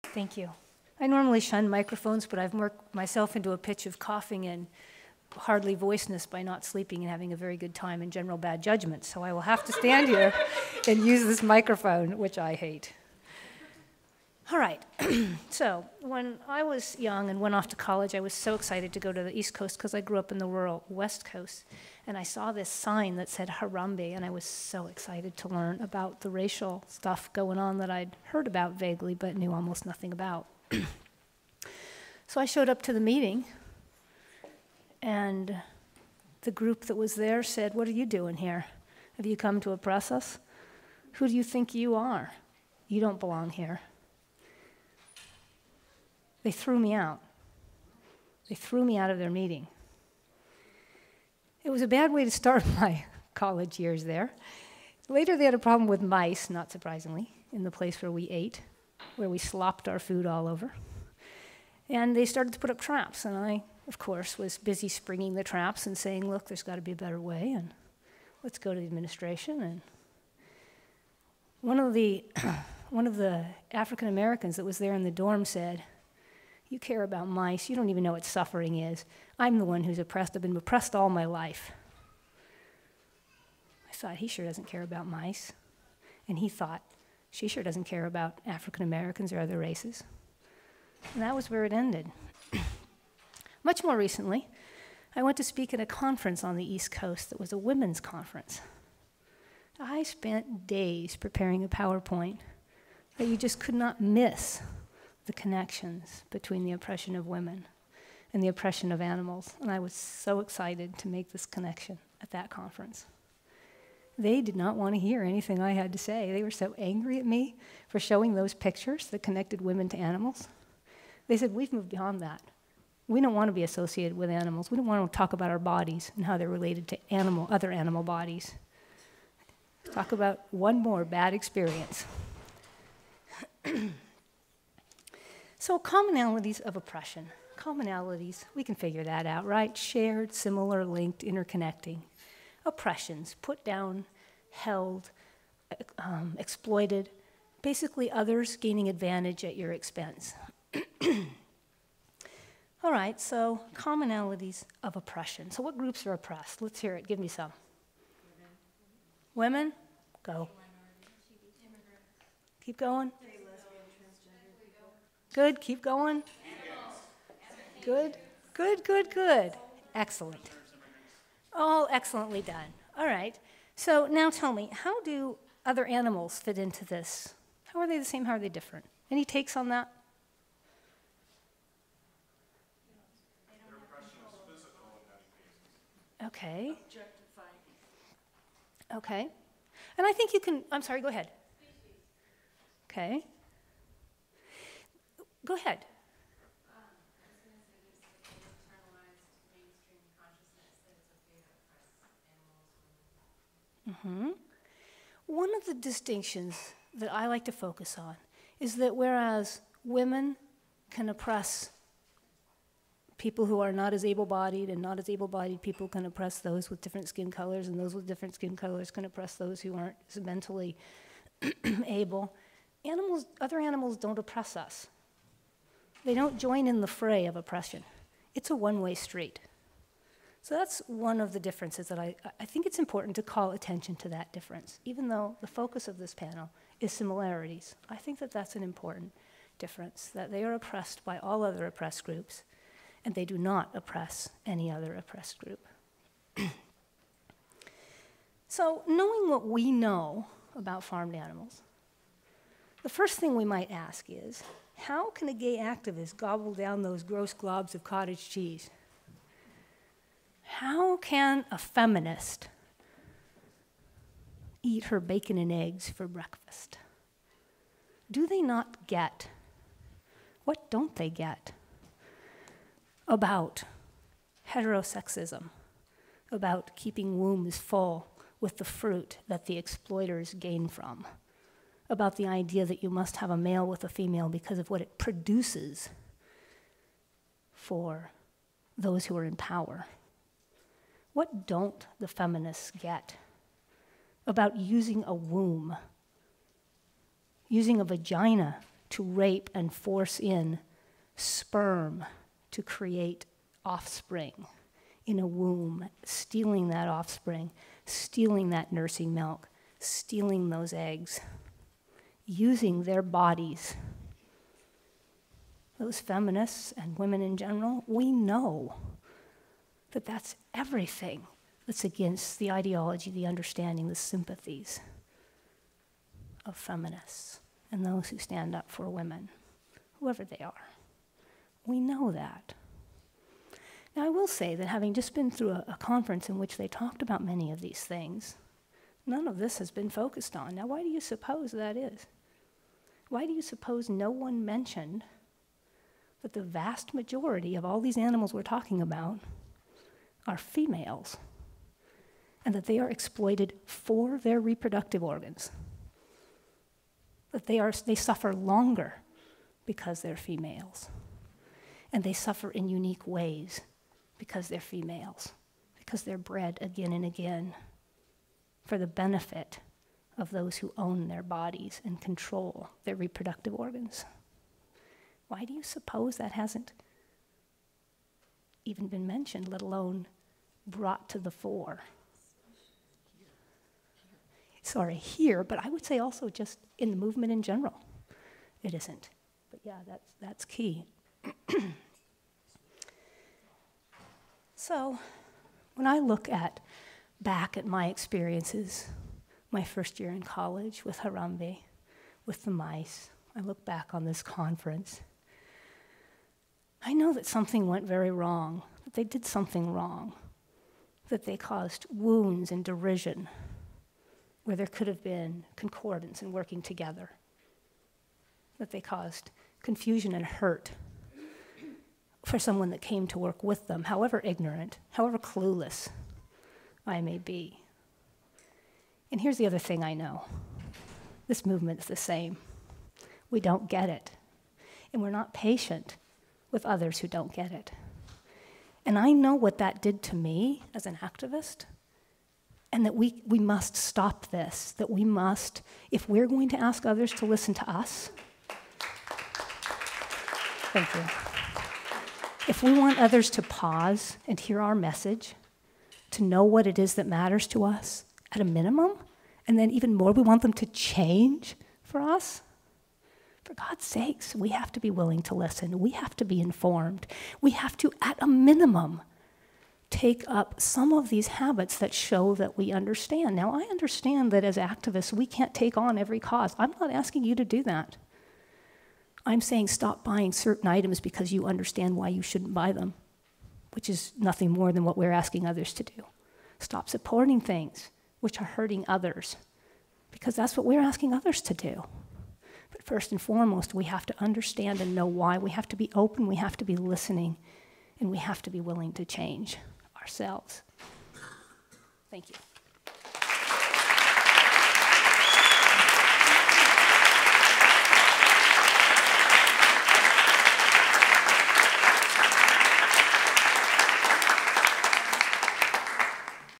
Commonalities of Oppression - Lisa Kemmerer
Featured at the the Animal Rights 2012 National Conference organized by Farm Animal Rights Movement (FARM), a 501(c)(3) tax-exempt organization.